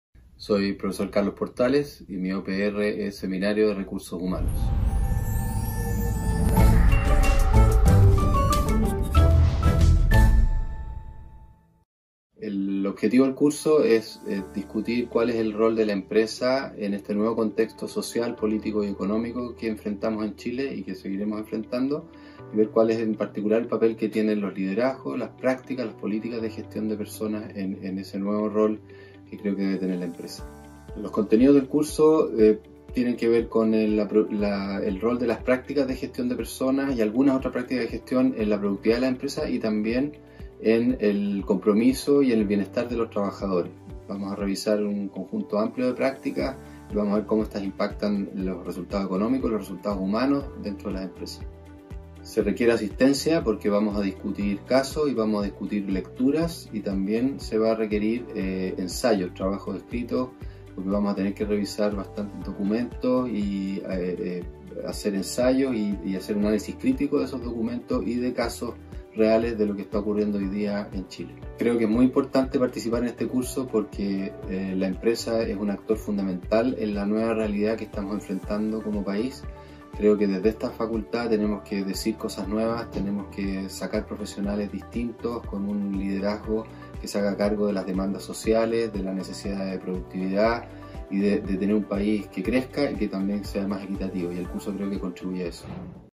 EAA349B - "Seminario de dirección de Recursos Humanos"
OPR 300
Dictado por Carlos Portales
Requisitos: EAA240A Ó EAA2110